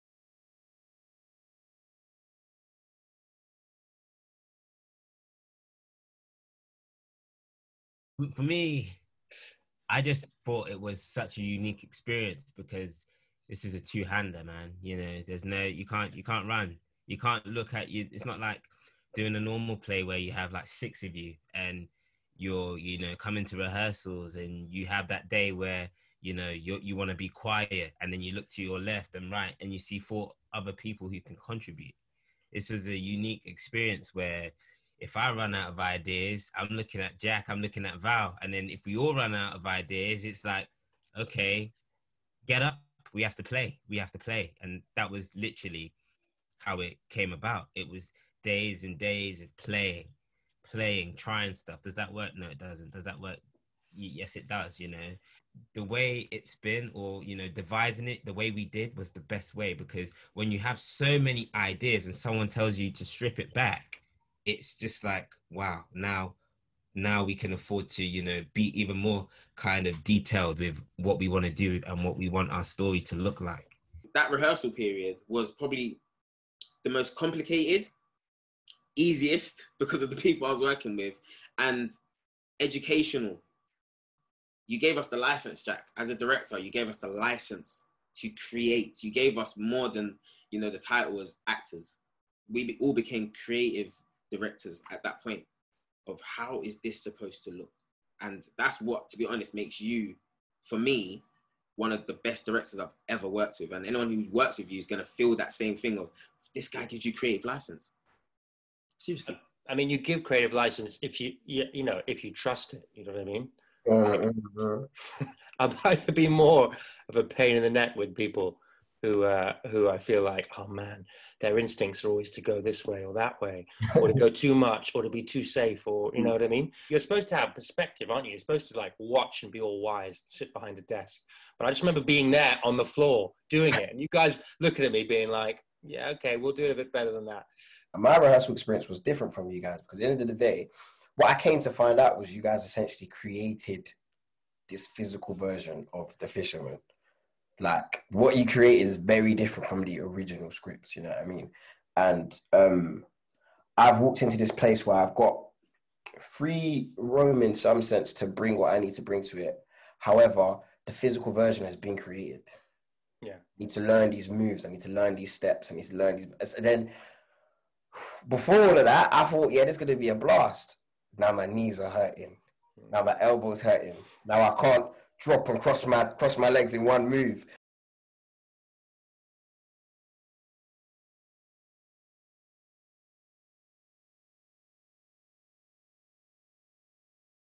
The Fishermen Rehearsals
Our award-winning production of The Fishermen by Chigozie Obioma, adapted by Gbolahan Obisesan has been produced for BBC Radio 3. In this interview Director Jack McNamara discusses the rehearsal process of the stage production with actors Valentine Olukoga, Michael Ajao and David Alade.

The recording, being broadcast Sunday 4 April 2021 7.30pm, will feature the original stage cast of Valentine Olukoga (winner of Best Actor at the Black British Theatre Awards 2020 and Stage Award for Outstanding Performances 2018), Michael Ajao (winner of Stage Award for Outstanding Performances 2018) and David Alade  - who took over the role originally played by Ajao - bringing all three actors together for the first time in this exclusive recording.

Based on the 2015 Man Booker Prize-shortlisted novel The Fishermen by Chigozie Obioma, Radio 3 will debut this powerful allegory of brotherhood, vengeance and fate, in a new adaptation by award-winning playwright Gbolahan Obisesan.

The Fishermen is a co-production between Naked Productions Ltd and New Perspectives, commissioned for BBC Arts and Radio 3 by Matthew Dodd.

Producer Polly Thomas
Director Jack McNamara
Composer Adam McCready
Sound Designer Paul Cargill
Executive Producer Eloise Whitmore
Assistant Producer Ravelle-Sadé Fairman.